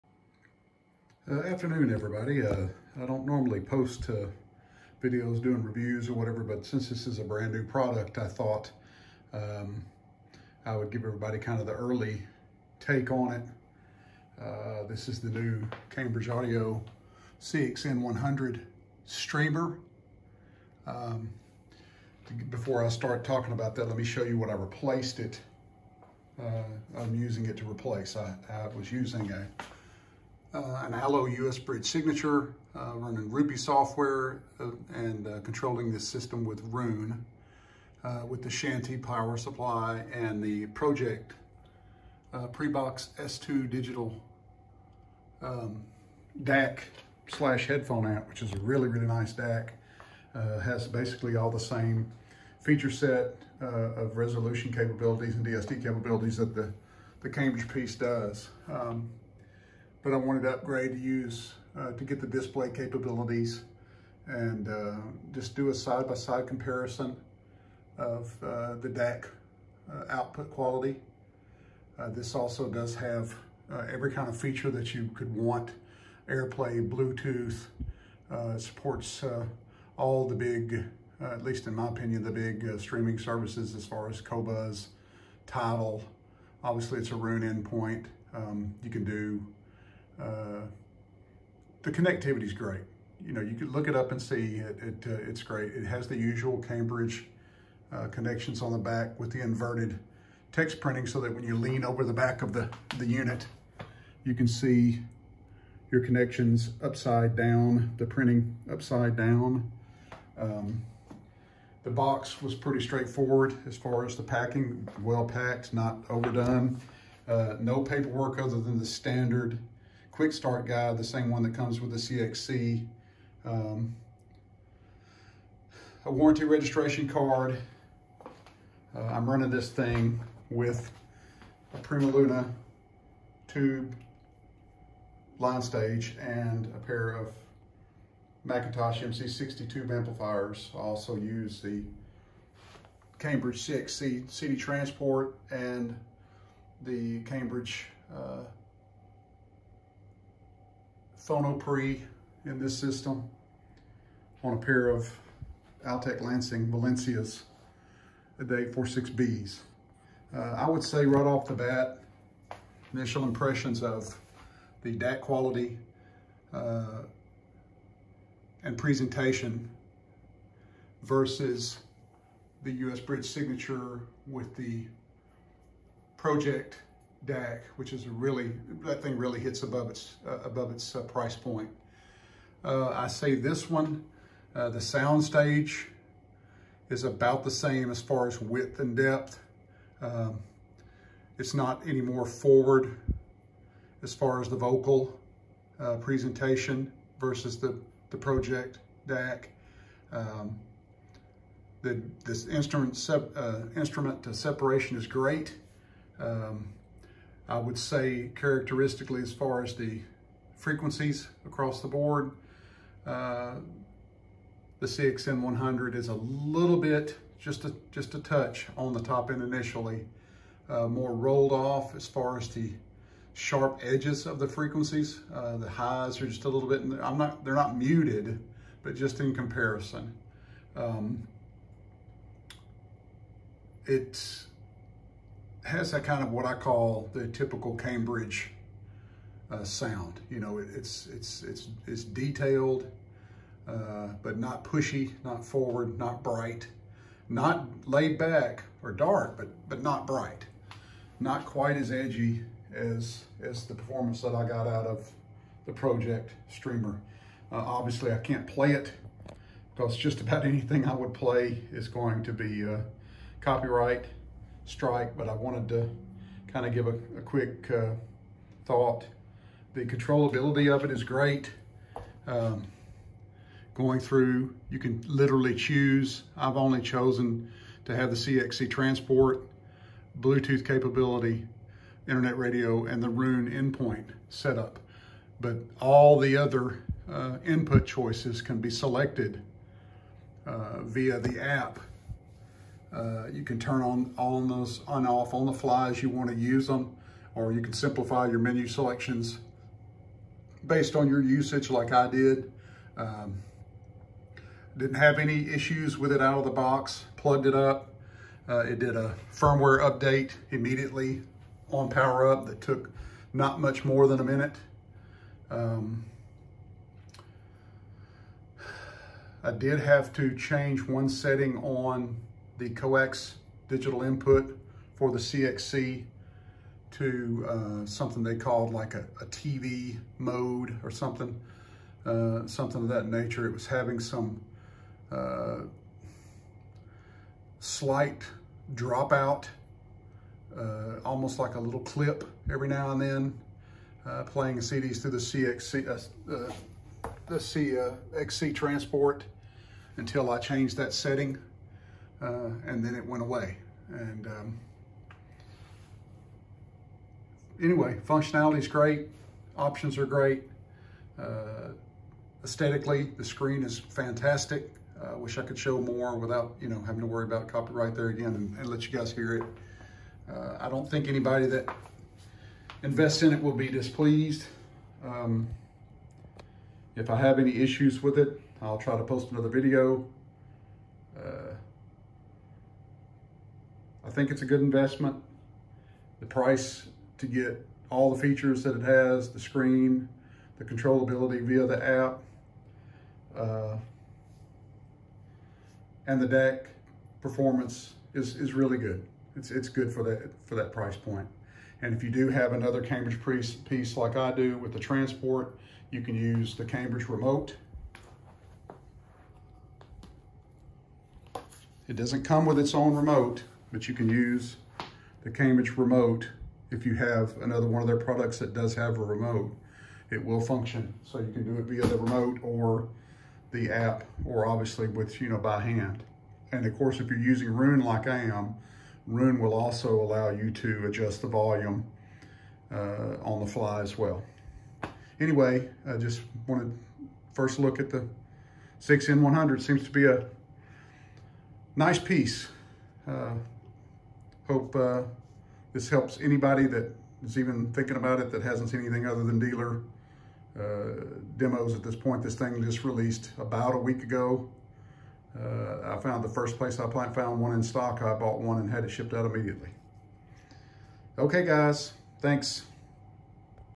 Cambridge CXN100 review
Initial install and test of the new Cambridge Audio CXN100 streamer.